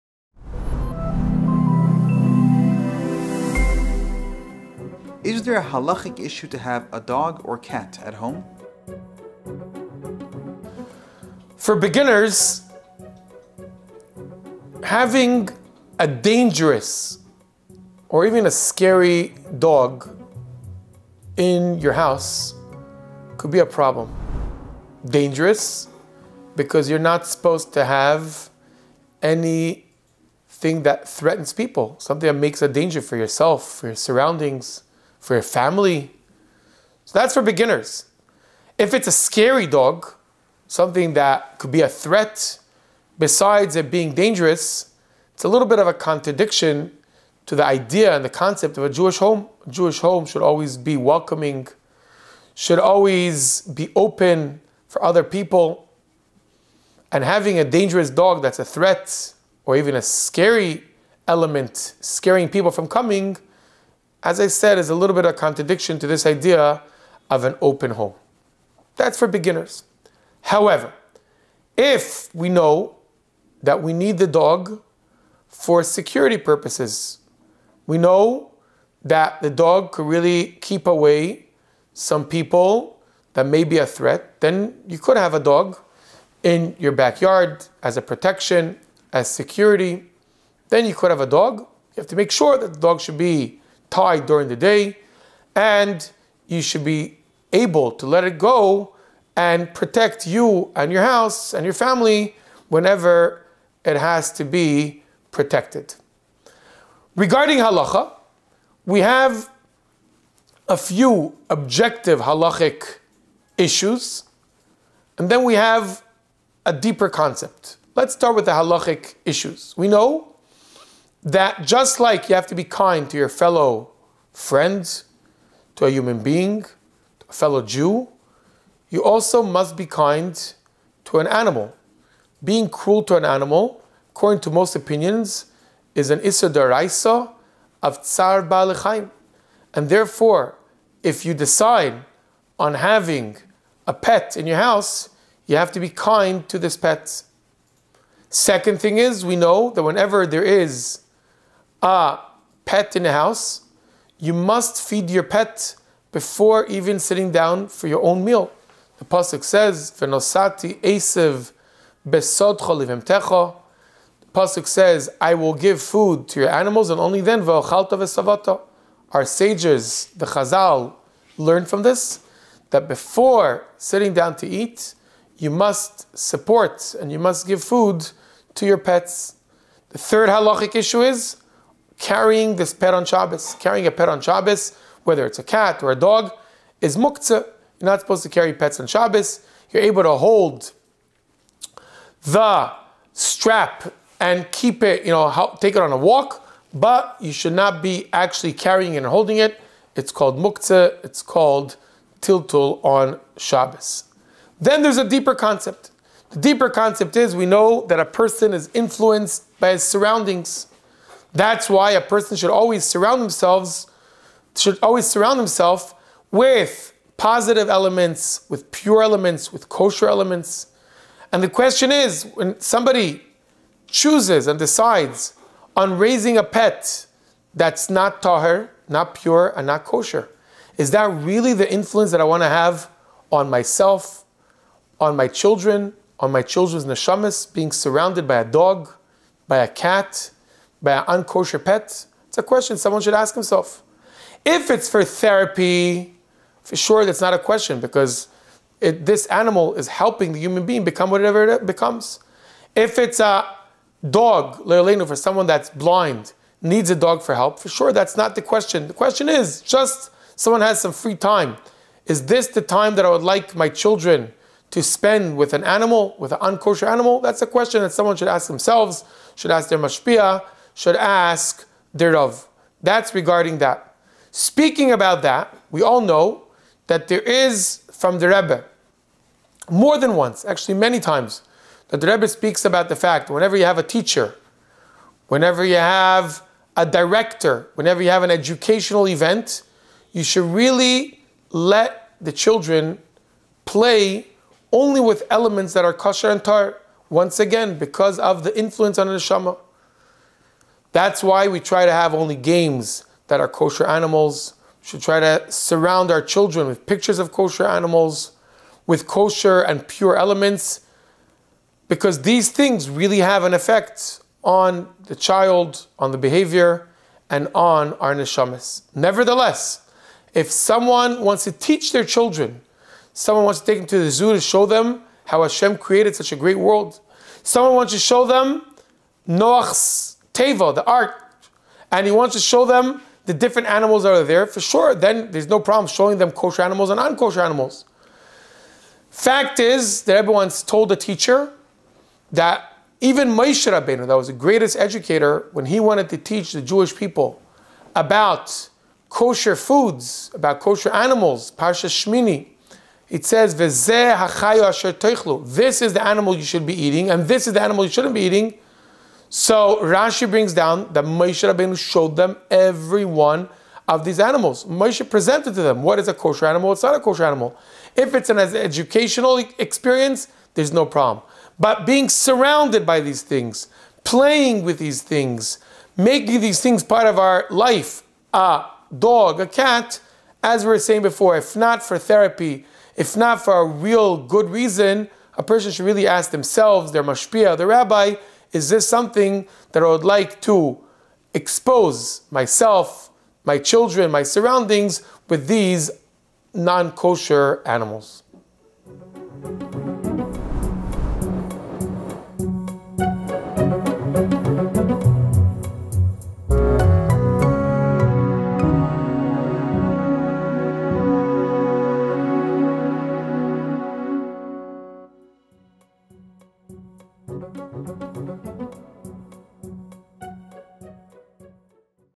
Is there a Halachic issue with having a dog or a cat at home?
Stump the Rabbi - Rabbi Shlomo Goldfarb

This project was made possible by

682